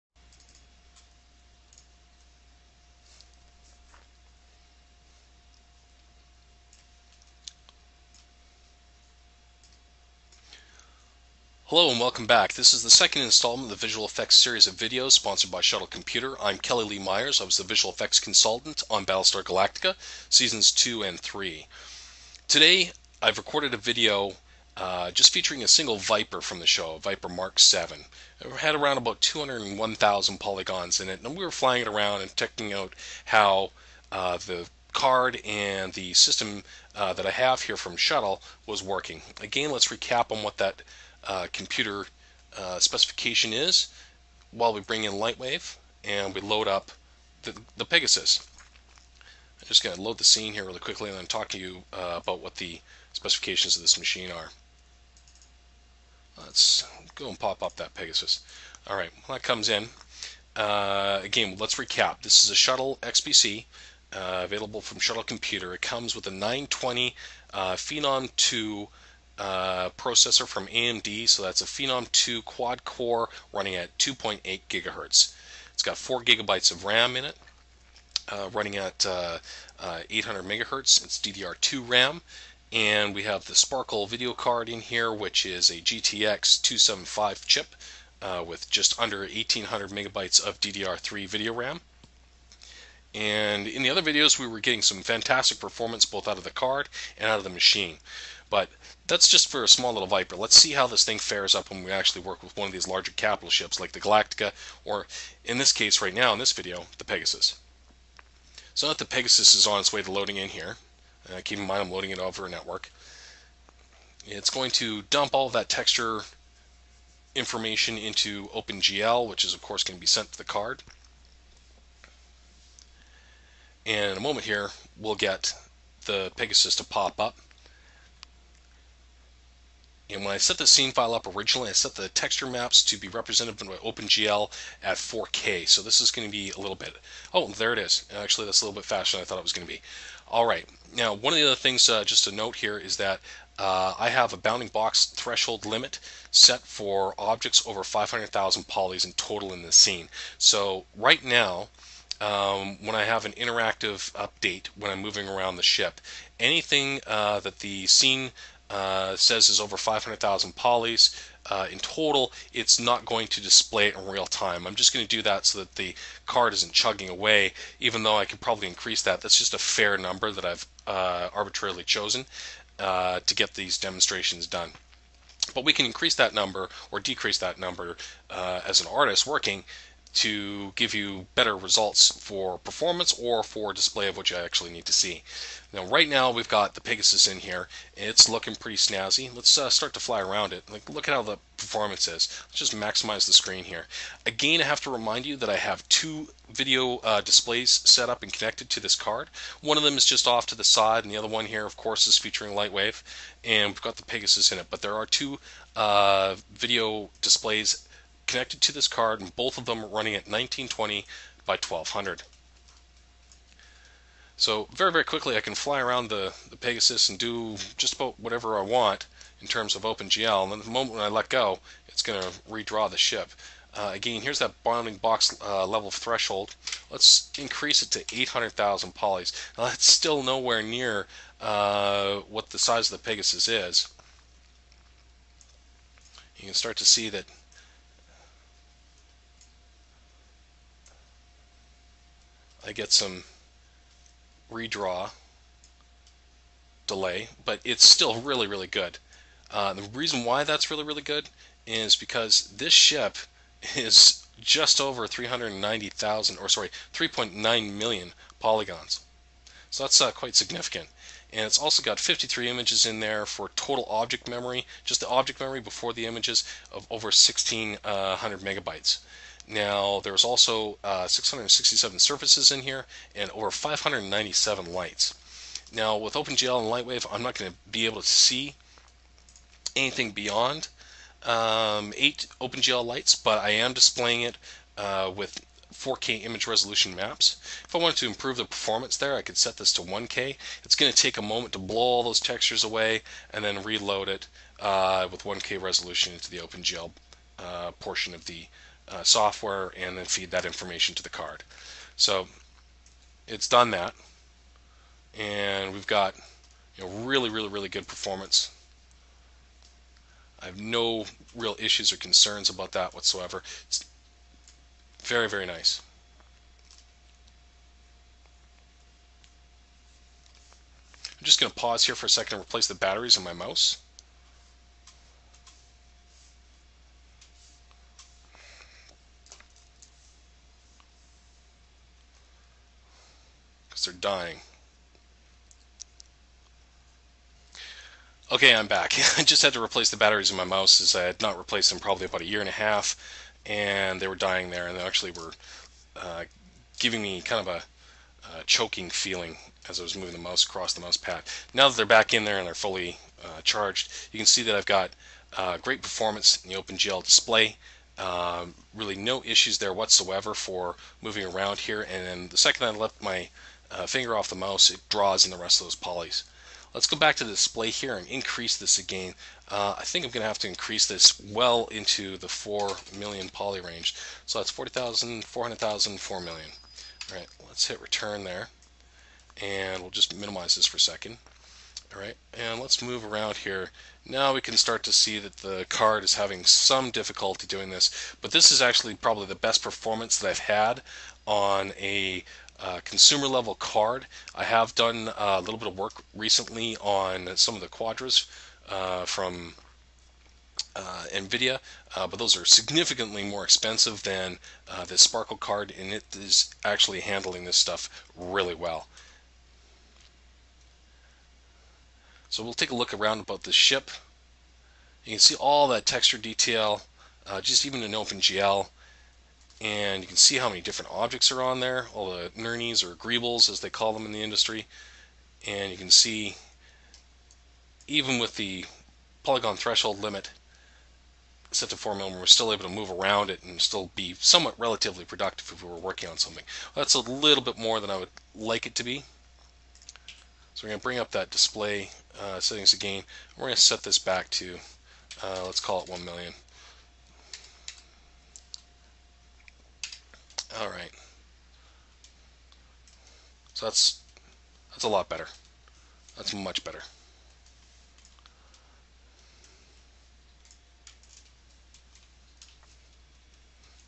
Battlestar Galactica Lightwave Demo Pegasus Video One
In this series of videos I push the performance of the Shuttle XPC (Phenom II 920 with 4GB ram and a sparkle GTX275 video card) using Lightwave3D 9.6 64bit edition.